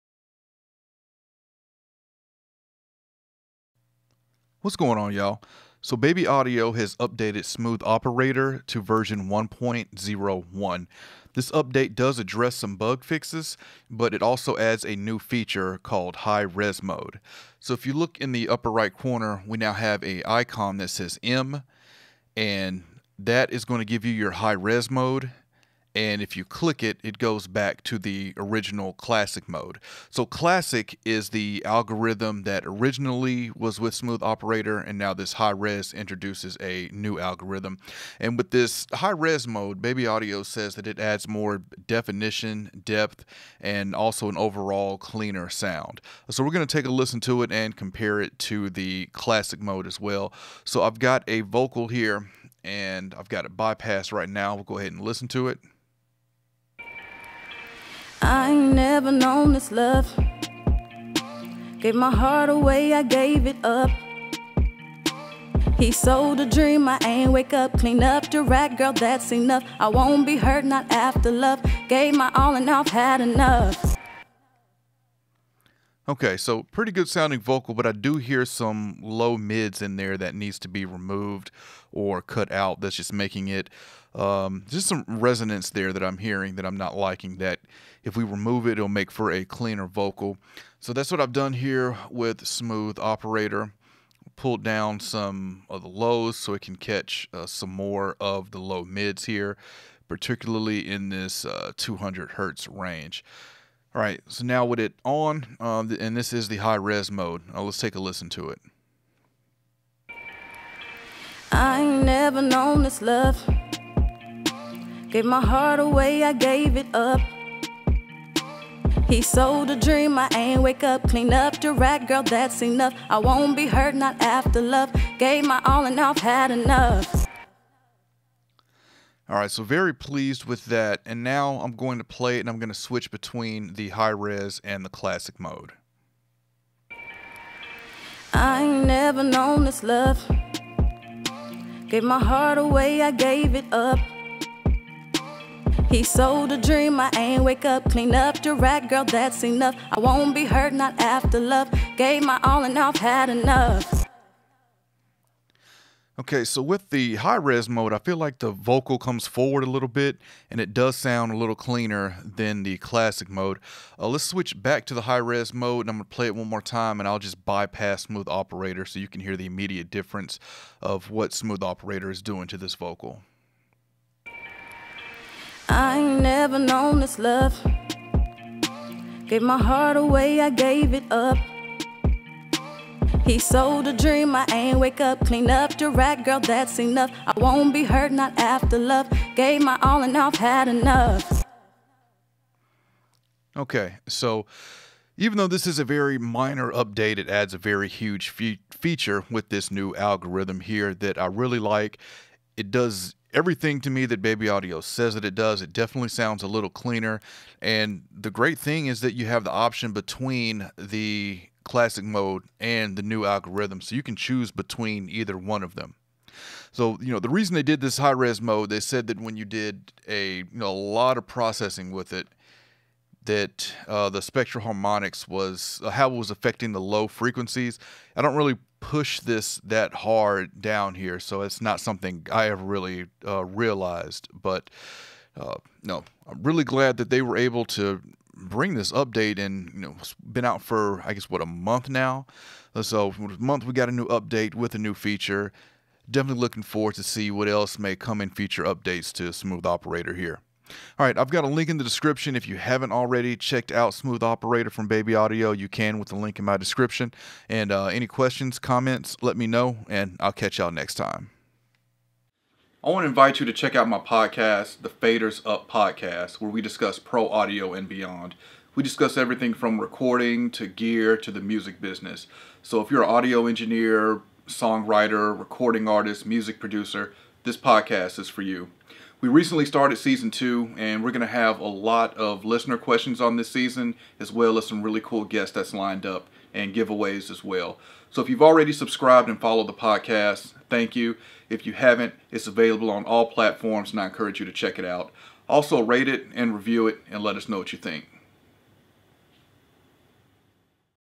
Baby Audio Smooth Operator new Hi Res Mode
This is a look at the new hi res mode in smooth operator

Also check out the Faders Up Podcast